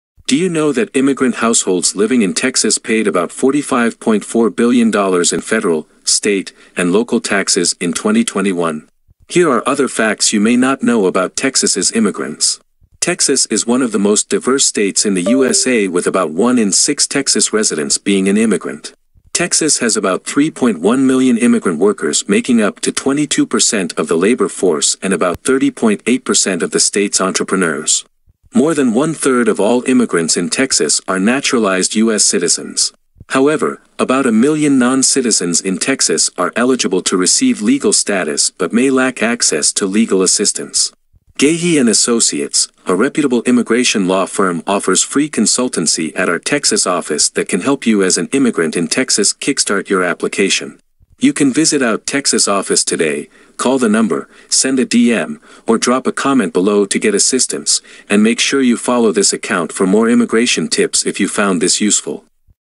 Interesting facts you may not know about Texas' immigrants.
About one million non-citizens in Texas are eligible to receive legal status but may lack access to legal assistance. Take advantage of FREE CONSULTATION with our immigration lawyers at our Texas office today.